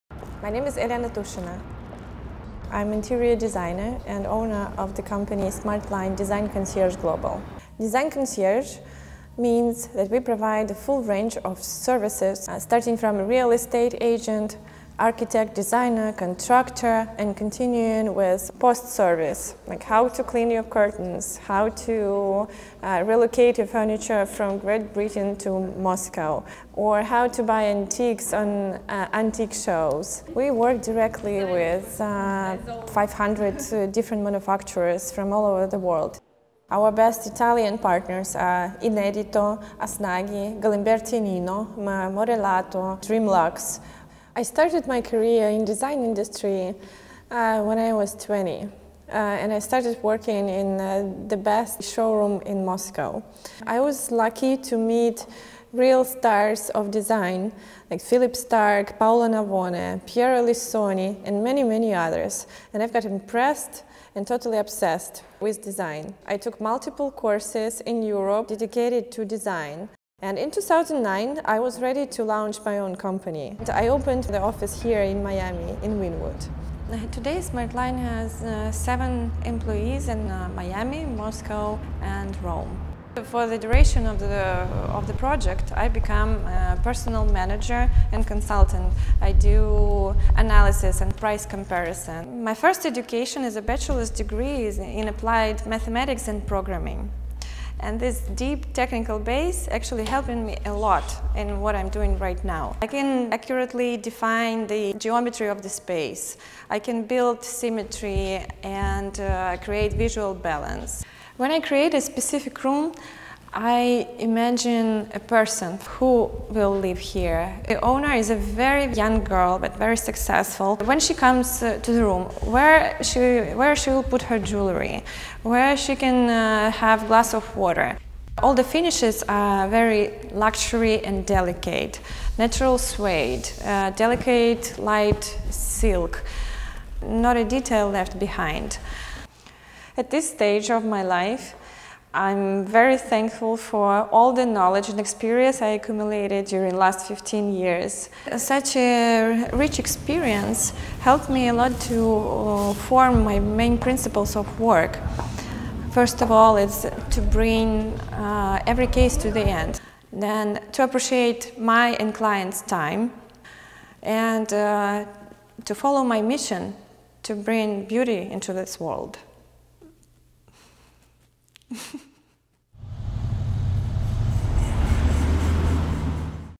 Elena Tushina, Interior Designer
Interior designer Elena Tushina talks about her career, and the company she founded in 2009, Smartline.